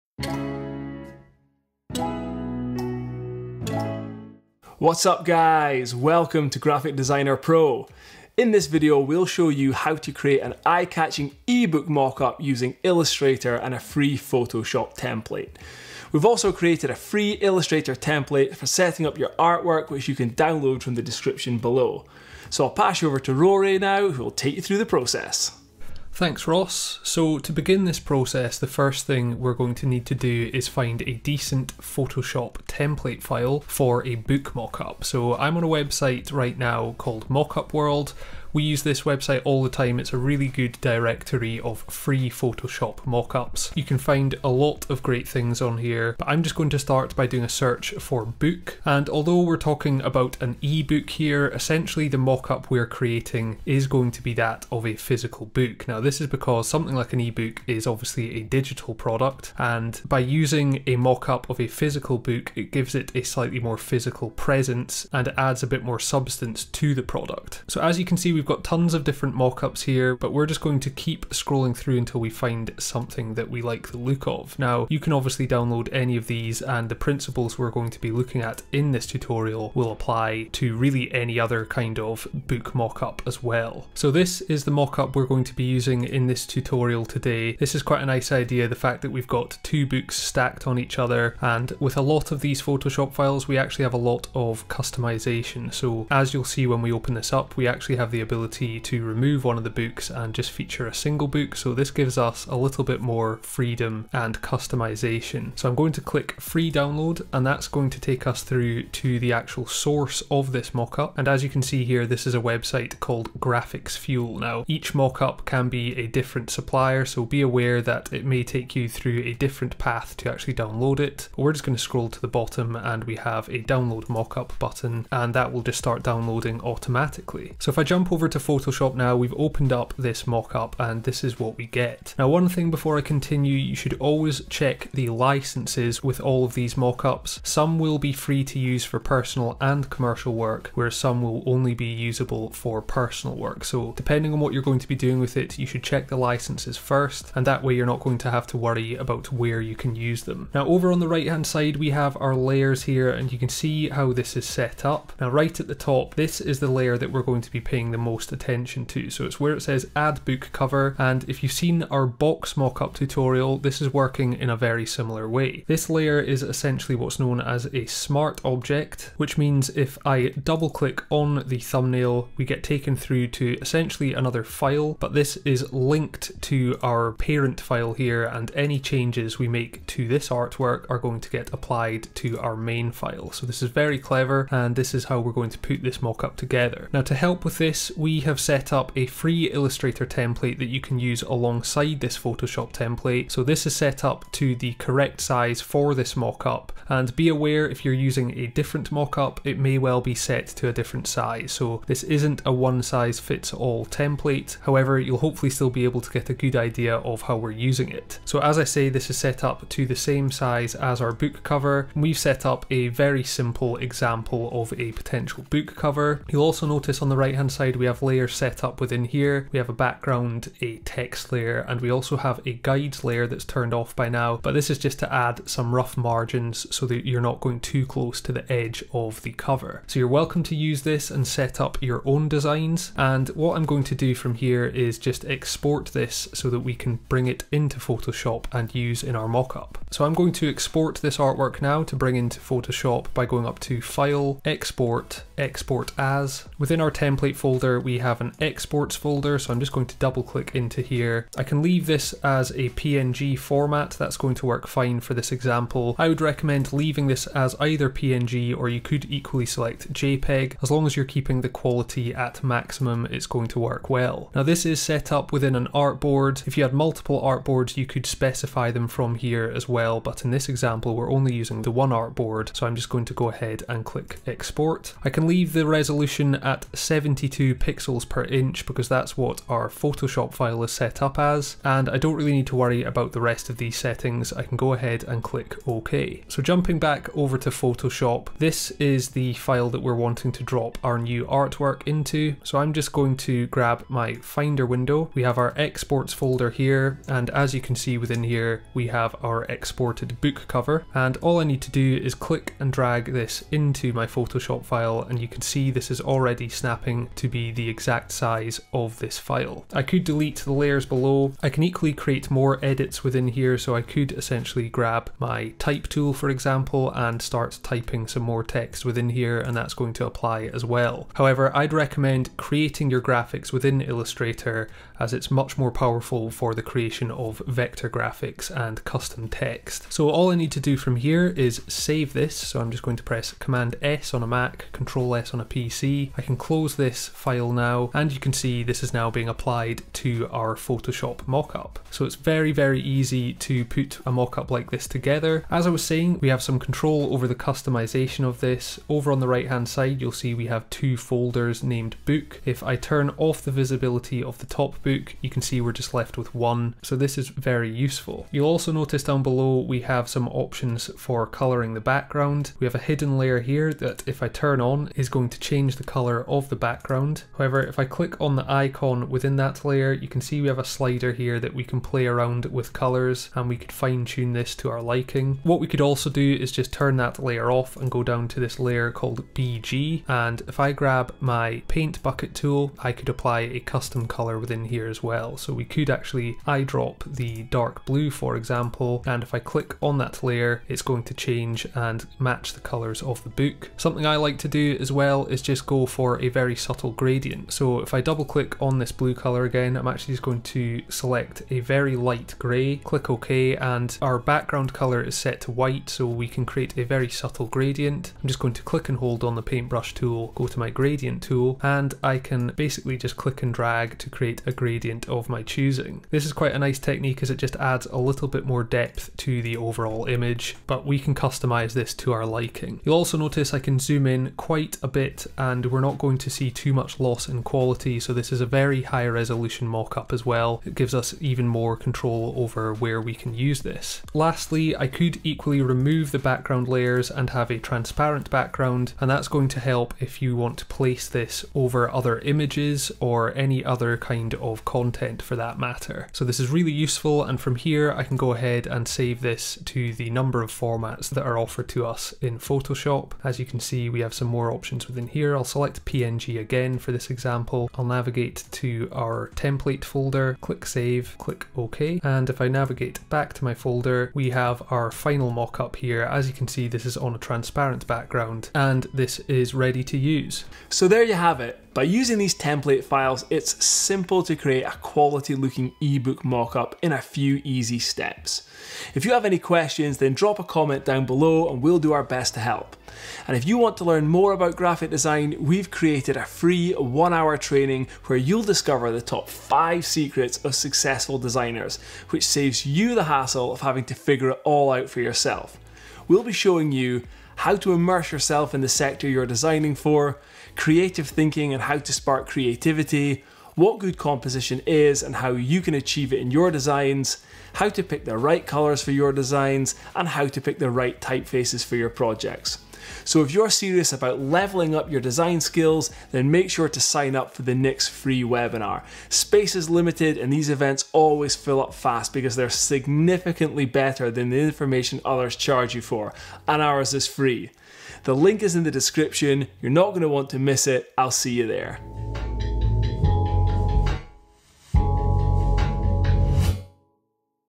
How to Create an Ebook Mockup • Adobe Photoshop & Illustrator Tutorial
- SORRY TEMPLATES NO LONGER AVAILABLE FOR DOWNLOAD BUT ENJOY THE FREE CONTENT - 👉 FREE TEMPLATE DOWNLOAD

In this video we’ll show you how to put together an ebook mockup using Adobe Photoshop and Illustrator.